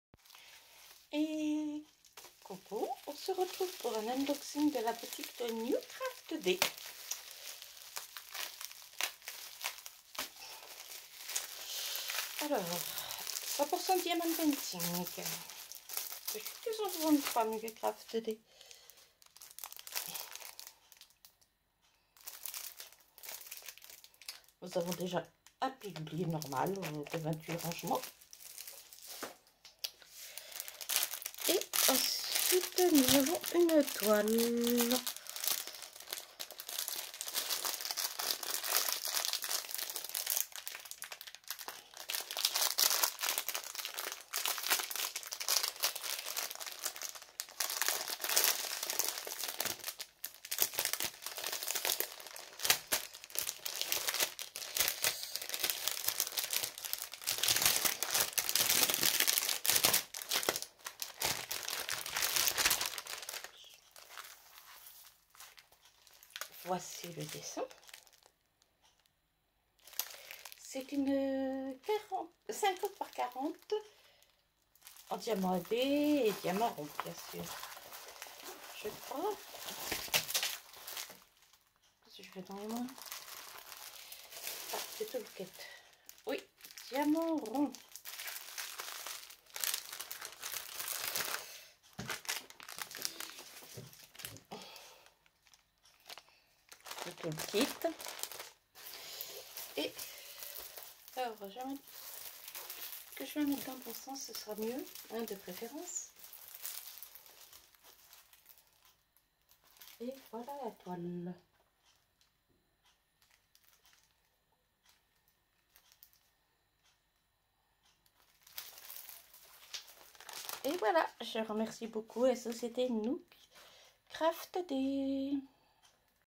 unboxing diamond painting newcraftday
cc tout le monde voici un unboxing de la boutique newcraftday
merci a eux

lien des objets